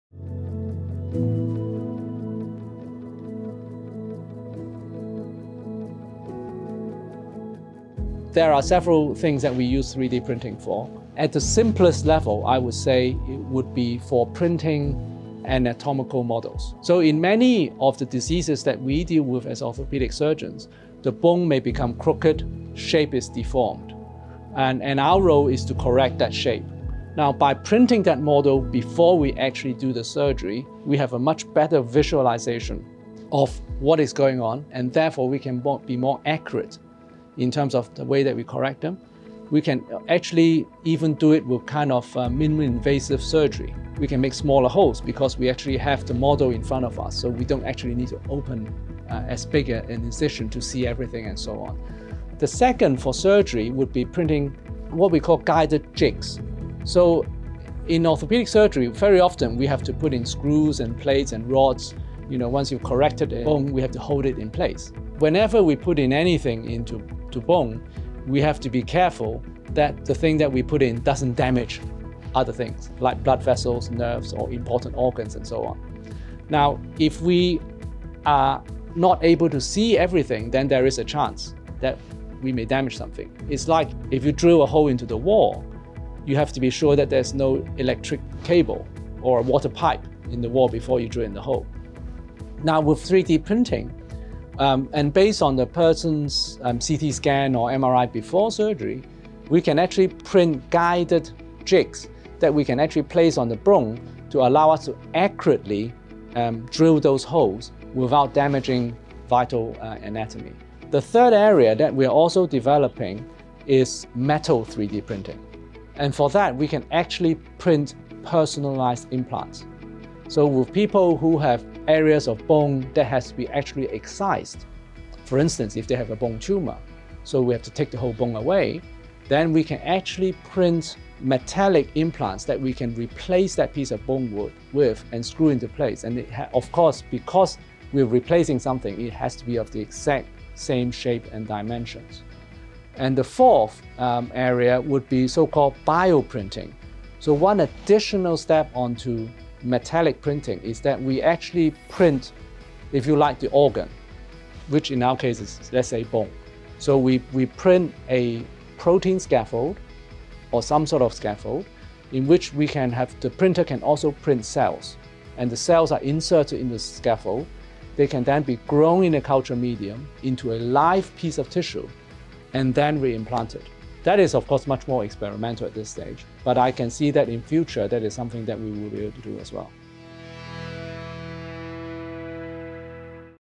How is 3D Printing Used in Orthopaedics and Surgery?
Professor Kenneth Cheung, head of the Department of Orthopaedics and Traumatology at HKUMed explains how 3D printing is being used by surgeons to be more accurate during procedures.